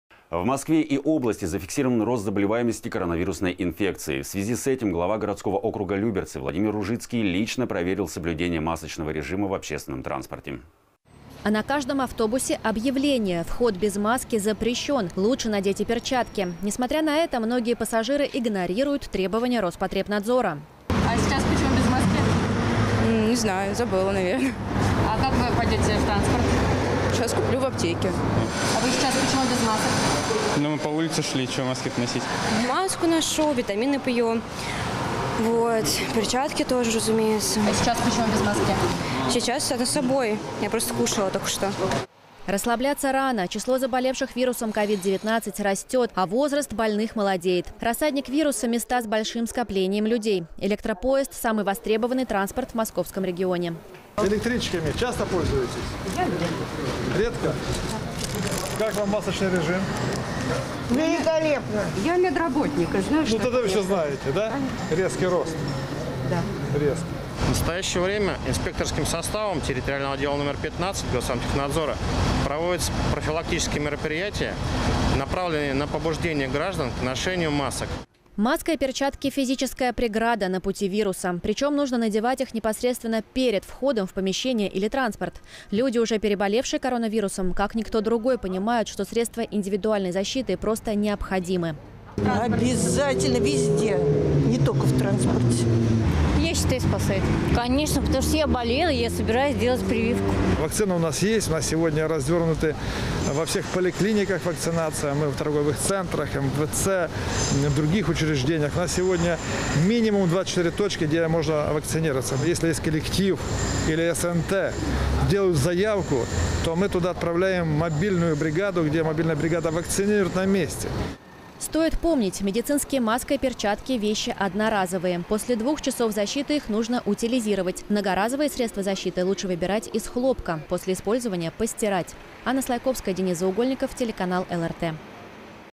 Без маски не входить!
В Москве и области зафиксирован рост заболеваемости коронавирусной инфекцией. В связи с этим глава городского округа Люберцы Владимир Ружицкий лично проверил соблюдение масочного режима в общественном транспорте.

 «Пожалуйста, не снимайте средства защиты до выхода со станции».

А на каждом автобусе - объявление - «Вход без маски запрещен». Лучше надеть и перчатки. Несмотря на это многие пассажиры игнорируют требования Роспотребнадзора.

«- А сейчас почему без маски? – Не знаю… Забыла, наверно. – А как Вы пойдете в транспорт? – Сейчас куплю в аптеке».

« – Вы сейчас почему без масок? – Ну мы по улице шли, что маски то носить…»
«Маску ношу, витамины пью. Перчатки тоже, разумеется. – А сейчас почему без маски? – Сейчас? Она с собой. Я просто кушала только что».

Расслабляться рано. Число заболевших вирусом COVID-19 растет. А возраст больных молодеет. Рассадник вируса - места с большим скоплением людей. Электропоезд – самый востребованный транспорт в Московском регионе.

« - Электричками часто пользуетесь? – Я нет – Редко? Как Вам масочный режим? – Великолепно! Я мед. работник и знаю, что это такое. – Ну тогда все знаете, да?– Резкий рост! – Резкий!».

Иван Кузнецов, консультант Территориального отдела №15 Госадмтехнадзора МО:
«В настоящее время инспекторским составом территориального отдела №15 Госадмтехнадзора проводятся профилактические мероприятия, направленные на побуждение граждан к ношению масок».

Маска и перчатки – физическая преграда на пути вируса. Причем нужно надевать их непосредственно перед входом в помещение. Люди, уже переболевшие коронавирусом, как никто другой, понимают, что средства индивидуальной защиты необходимо использовать.

«Обязательно везде! Не только в транспорте».
«Я считаю, спасает. – Вы сами всегда пользуетесь? – Конечно, потому что я болела и я собираюсь делать прививку».

Владимир Ружицкий, глава г.о. Люберцы:
«Вакцина у нас есть. У нас сегодня развернуты во всех поликлиниках вакцинация. Мы в торговых центрах, МФЦ. В других учреждениях. У нас сегодня минимум 24 точки, где можно вакцинироваться. Если есть коллектив или СНТ, делают заявку, то мы туда отправляем мобильную бригаду, где мобильная бригада вакцинирует на месте».

Стоит помнить, медицинские маска и перчатки  – вещи одноразовые. После двух часов защиты их нужно утилизировать. Многоразовые средства защиты лучше выбирать из хлопка. Они должны плотно прилегать к лицу и рукам.